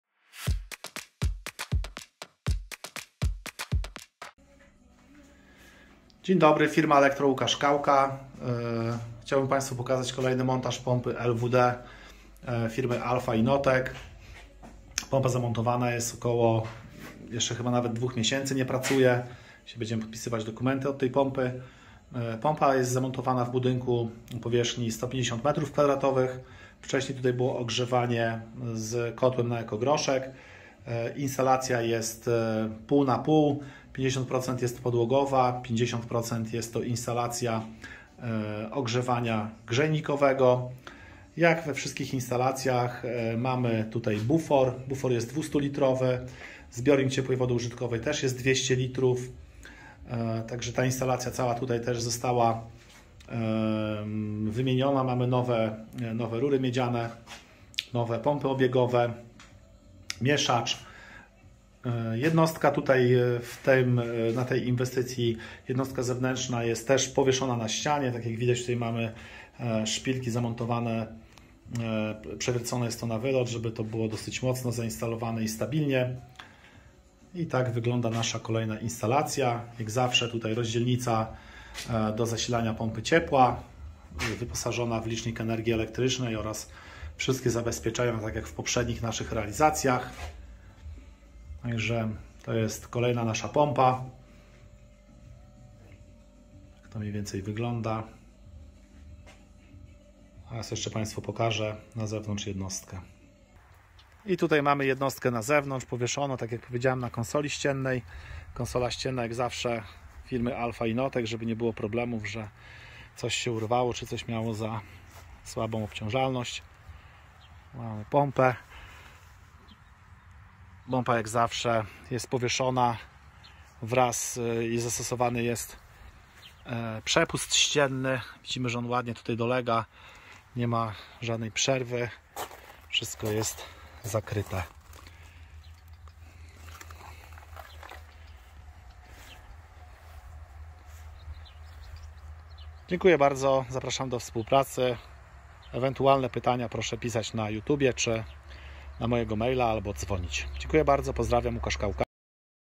Pompa ciepła LWD w instalacji CO 50% podłogówki i 50% grzejniki
Instalacja pc w domu z instalacja CO mieszana, podłogówka i grzejniki.